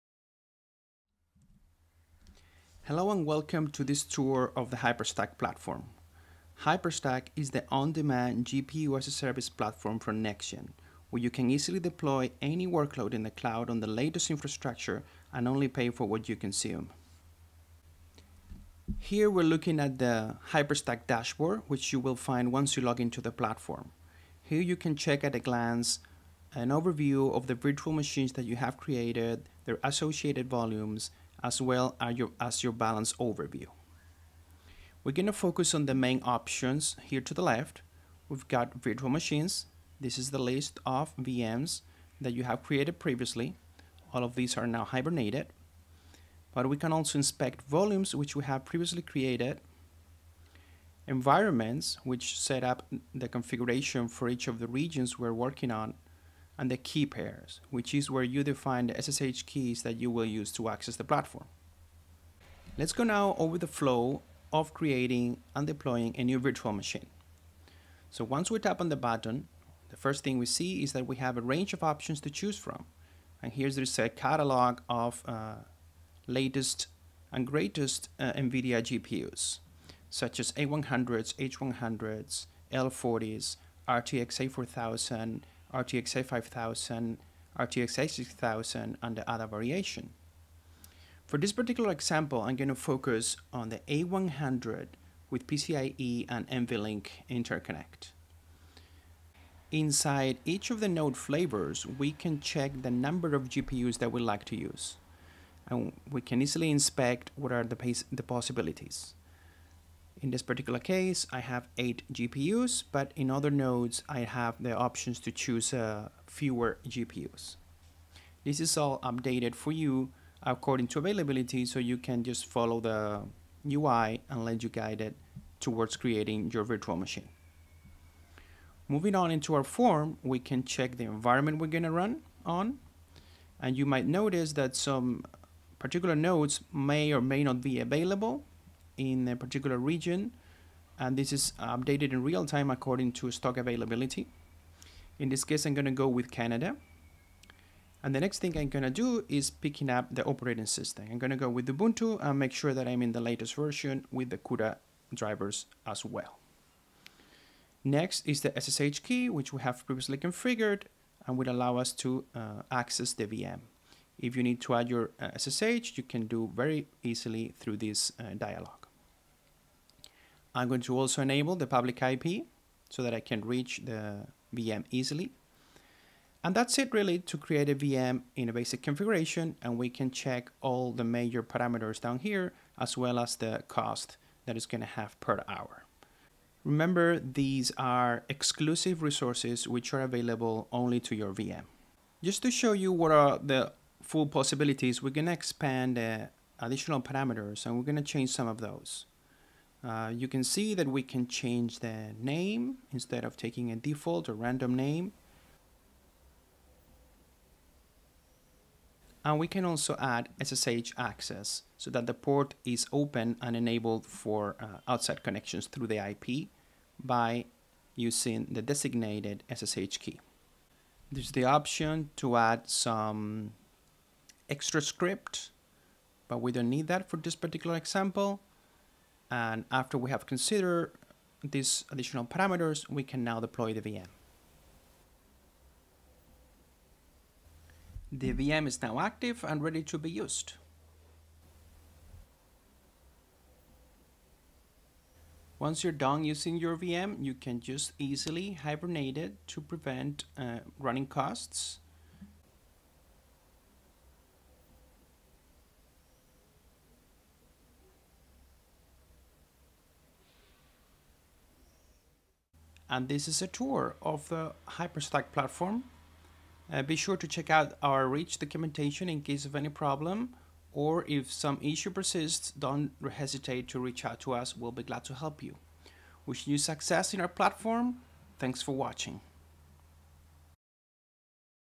Hyperstack GPU Cloud Platform Quick Tour!
Getting started on Hyperstack is easy and convenient.

Reserve powerful GPU-powered compute nodes and pay only for what you use.

In this short video we show you how to navigate the Hyperstack dashboard to manage the lifecycle of your virtual machines (VMs):

- create a VM from a broad selection of NVIDIA GPUs, such as NVIDIA RTX-A6000, L40, A100 or H100,

- define parametres for access, and,

- hibernate your VM when you are done, to avoid incurring extra costs.

Check out the extended version, which includes a full example of accessing the VM through the terminal and running a sample LLM inference workload,